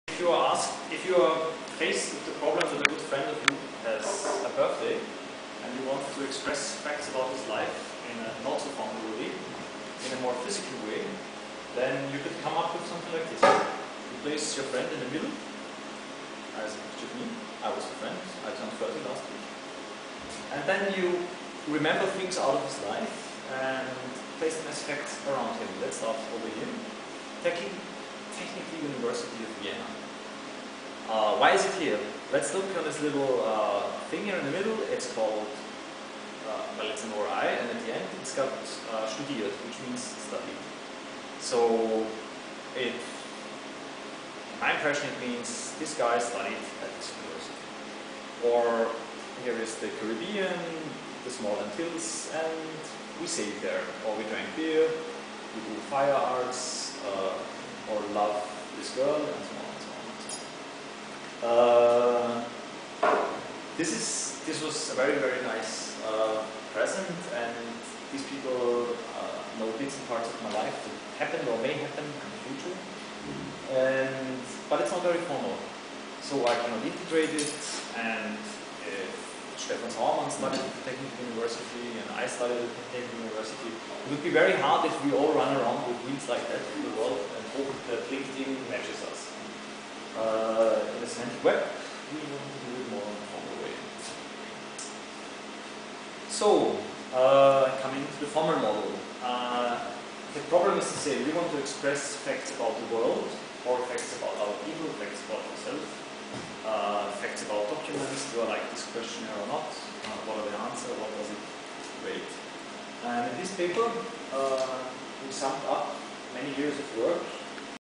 Explaining the Personal Semantic Web haptical
Explaining the theory using a haptical example. The demo material was given to me by Matth5w, Rosi, Chris, Bettina, Wolfgang, my old friends from university.